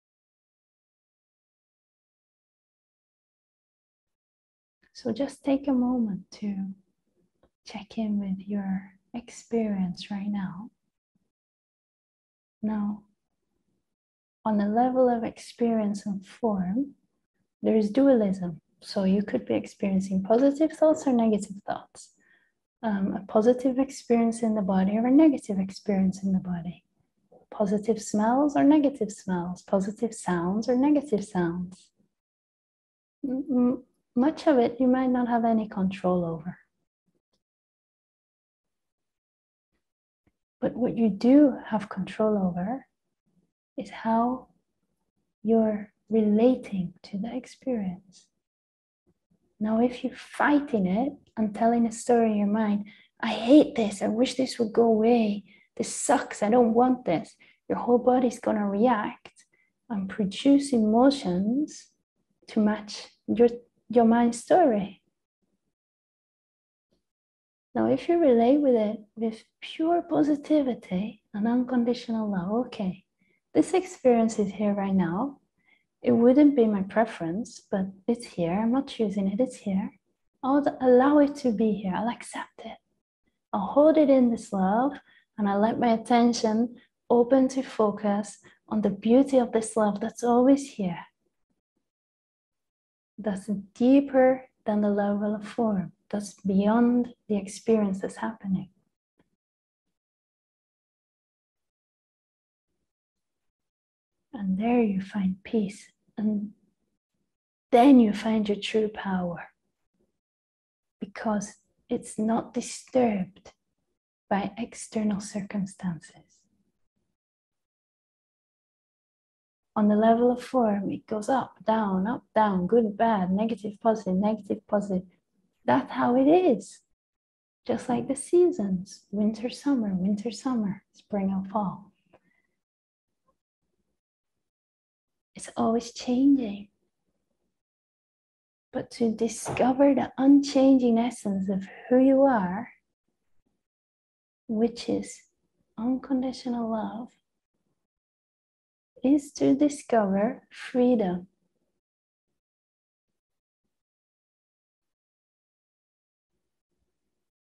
Freedom Beyond Forms
Experiences on the level of form are constantly changing but when we discover the depth of formless awareness we discover an eternal freedom, which is unchanging regardless of experience.

This is a clip from the open Zoom meeting on November 21st 2021.

Louise Kay holds group events and retreats online and around the world.